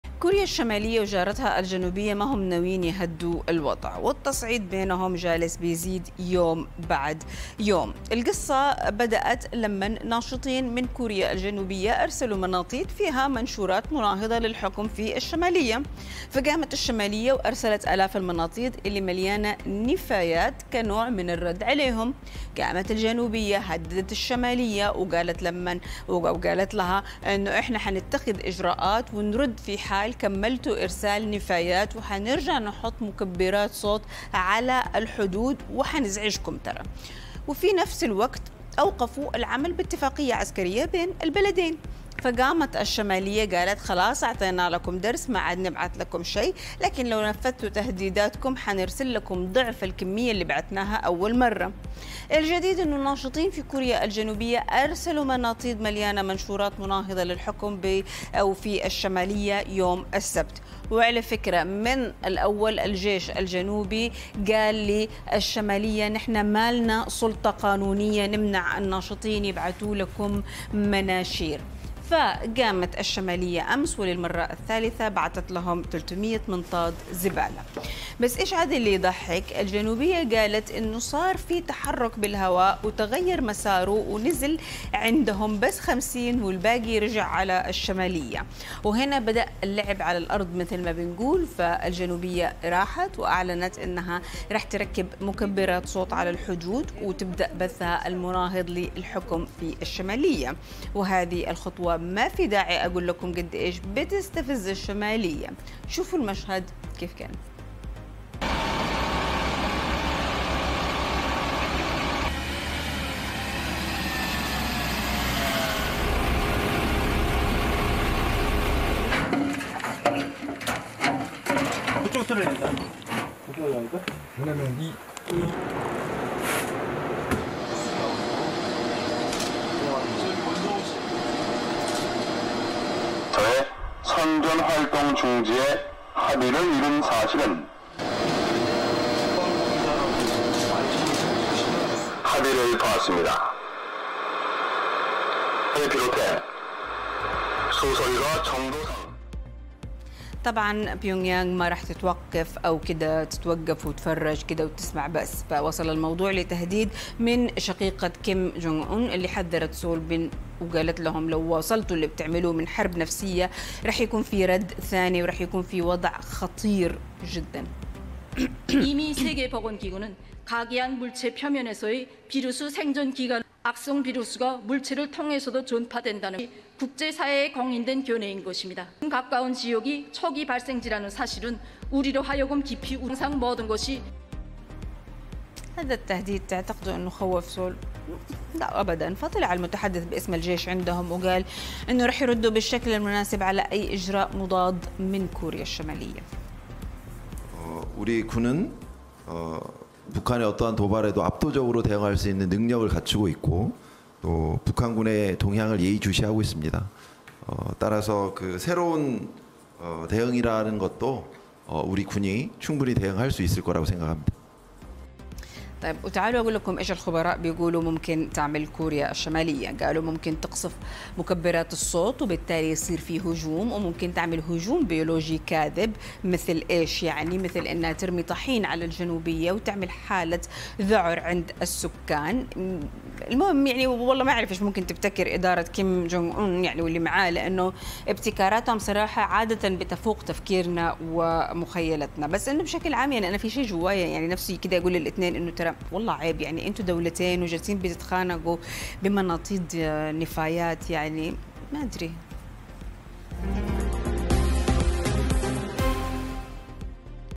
تفاعلكم | مناطيد كوريا الشمالية المليئة بالنفايات تضل طريقها وتعود من حيث أتت 😂
مستجدات حرب المناطيد بين الكوريتين.. أكثر من نصف مناطيد النفايات التي أرسلتها كوريا_الشمالية إلى جارتها الجنوبية تعود من حيث أتت ترد بفعل الرياح، وشقيقة الزعيم الكوري الشمالي تتوعد برد جديد على استفزازات كوريا_الجنوبية!

لمعرفة أحدث المستجدات.. عبر البث المباشر من هنا ⤵
وبإمكانكم متابعة العربية على منصات التواصل